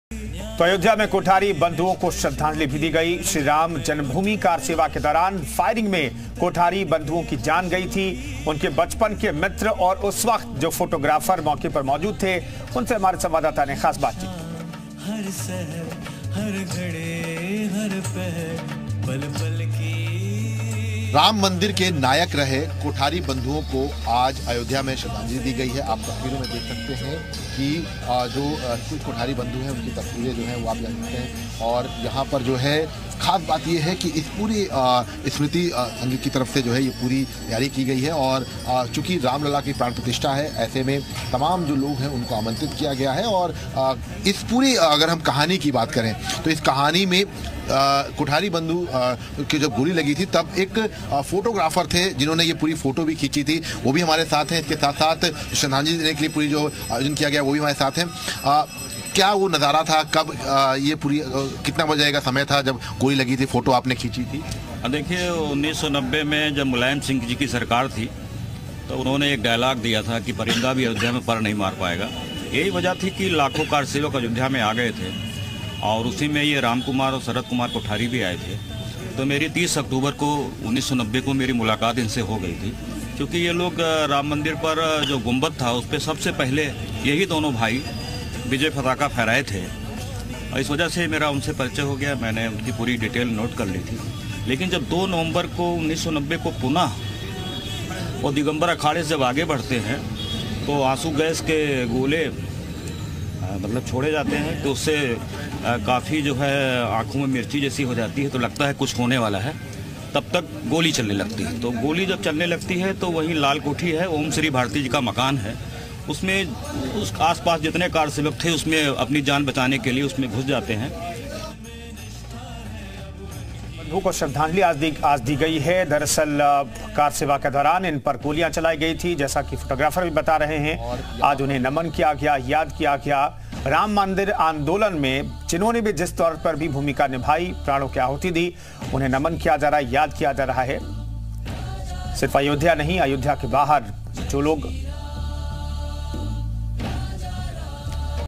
Ram Mandir Pran Pratishtha से पहले Ayodhya में दी गई Kothari Brothers को श्रद्धांजलि, देखें वीडियो
Ram Mandir Pran Pratishtha : कोठारी बंधुओं को किया गया याद, लोगों ने कोठारी बंधुओं को दी श्रद्धांजली, कारसेवा के दौरान गंवाई थी जान

Ram Mandir Pran Pratishtha: Kothari brothers were remembered, people paid tribute to Kothari brothers, who lost their lives during Karseva.